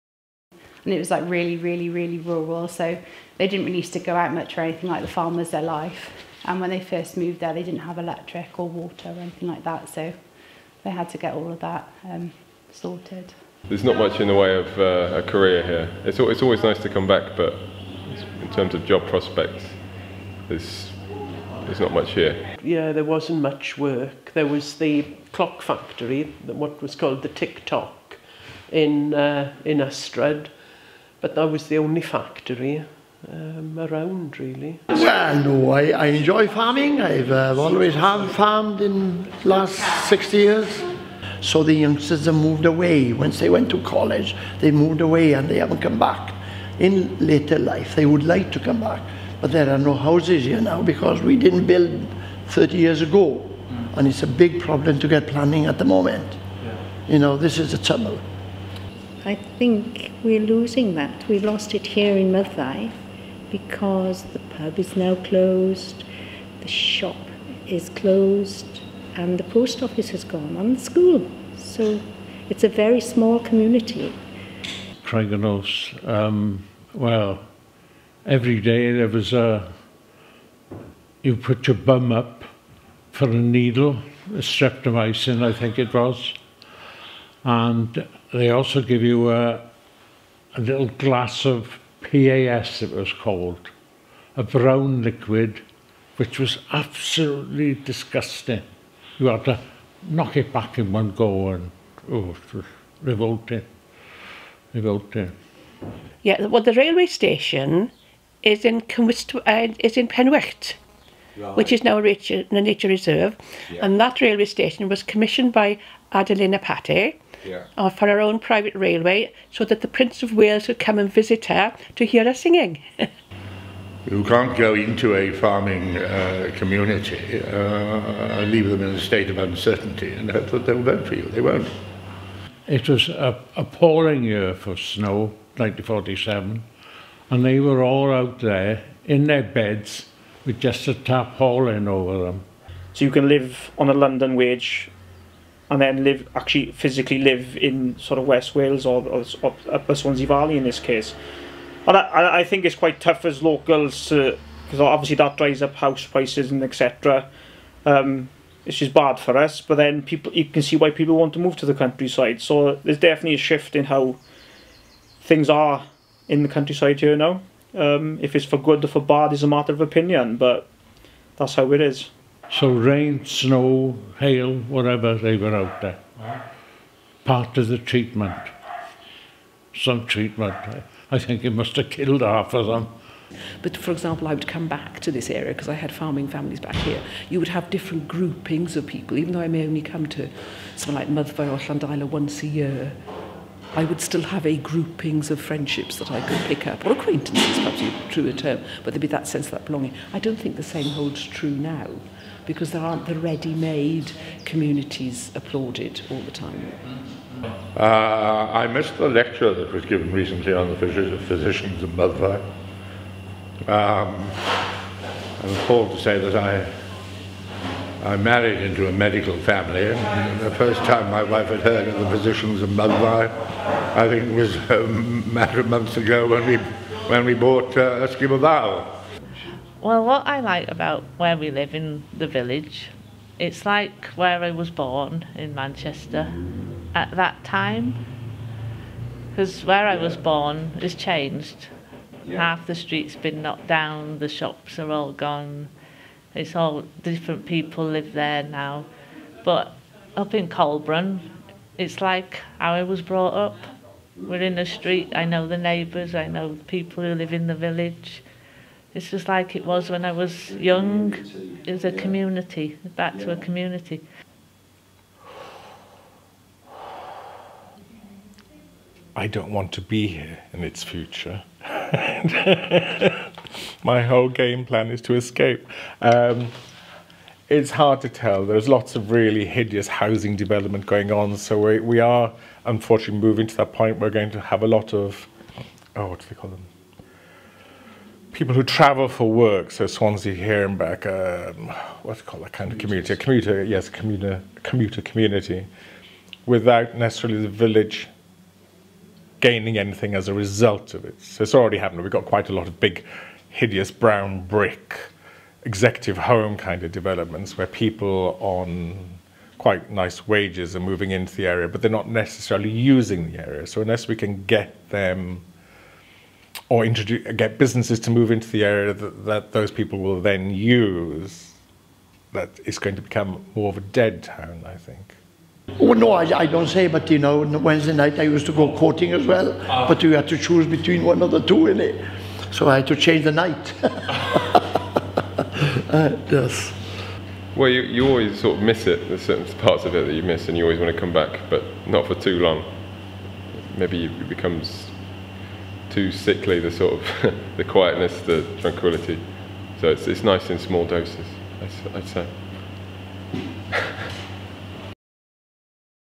TRAILER | Forces That Shape Our Communities
Short interview exerts from our current Heritage funded project 'Forces That Shape Our Communities'.